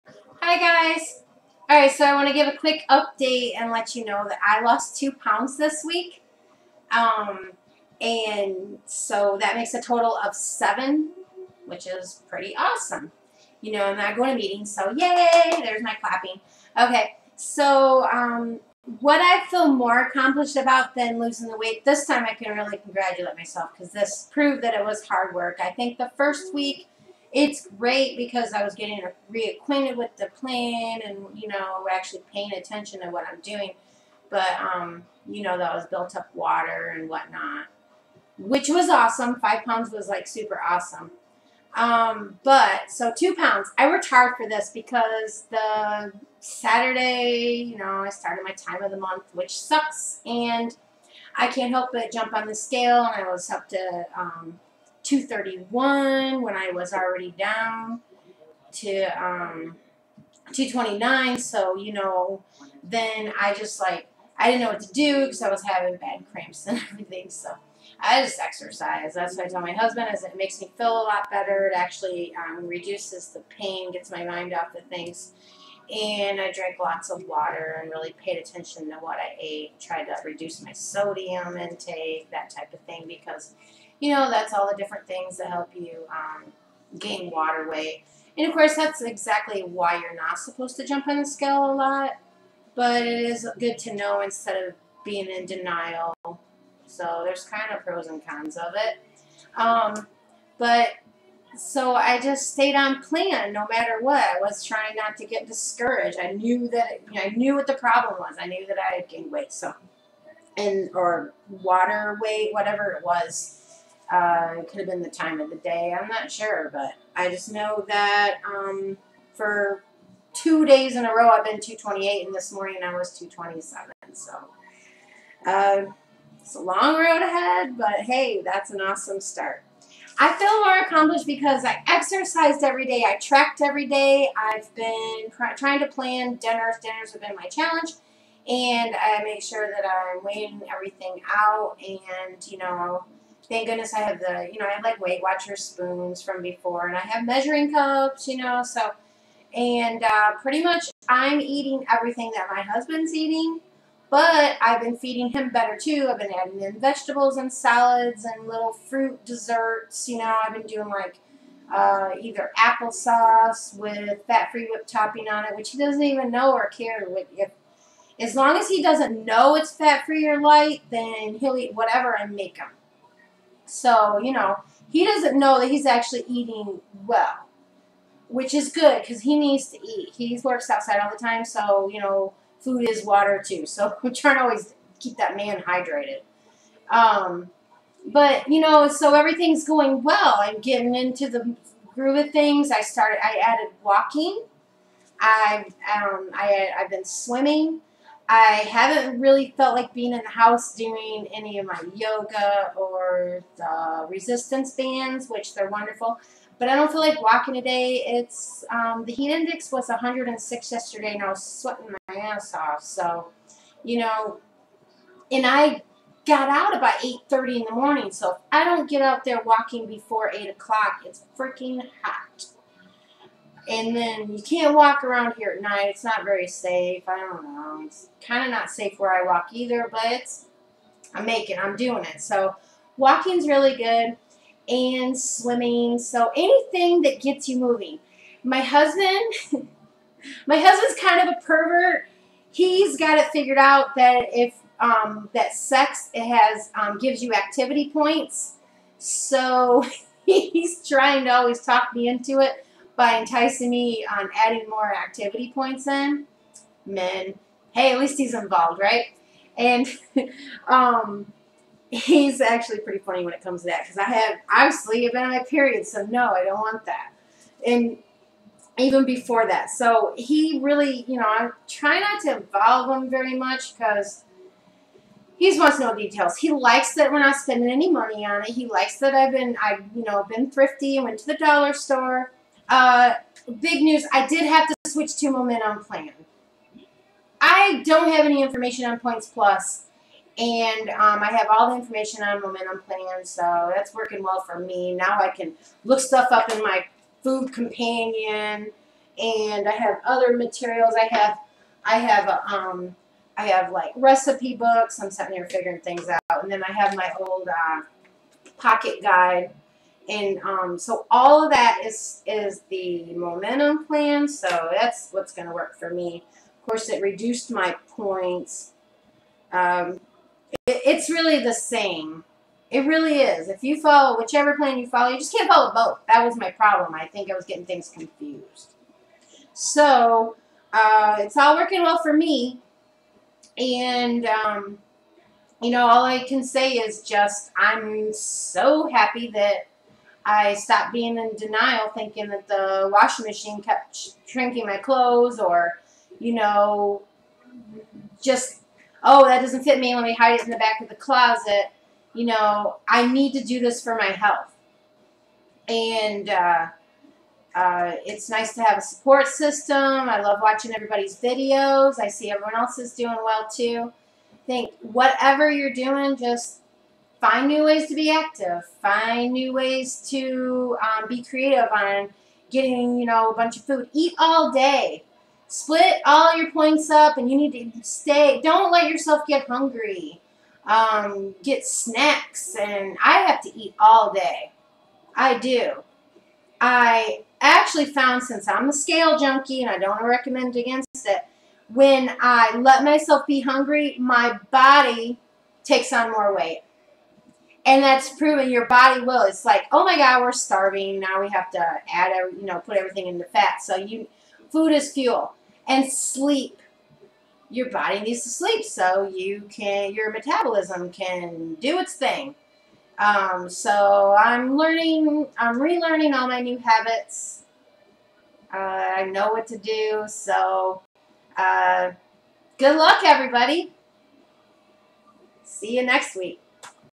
WW's @ home: Week 2/Day 15 227.6
I lost 2 lbs by moving more and paying attention to what I eat. That is a total of 7lbs so far. Yay me. I already feel better.